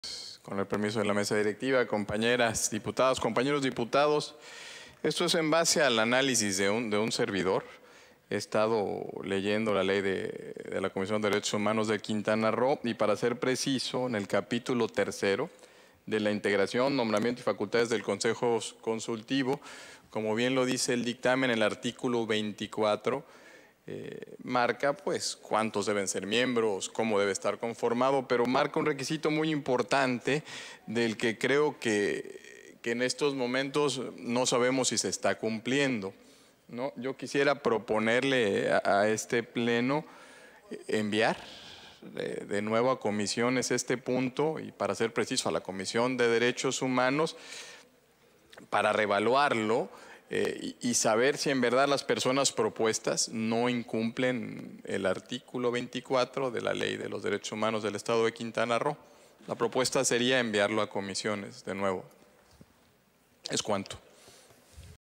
Intervención Dip. Guillermo Andrés Brahms González. PVEM (11/Diciembre/2023)
Intervención del Dip. Guillermo Andrés Brahms González, Presidente de la Comisión de Derechos Humanos de la XVIILegislatura, en relación al Dictamen que contiene la lista de personas aspirantes al cargo de Consejera o Consejero del Consejo Consultivo de la Comisión de los Derechos Humanos del Estado de Quintana Roo, que cumplen con los requisitos establecidos en el artículo 24 de la Ley de la Comisión de los Derechos Humanos del Estado de Quintana Roo.
Publicado en la Gaceta Parlamentaria Año II número 23 Ordinaria de fecha 28 de noviembre de 2023.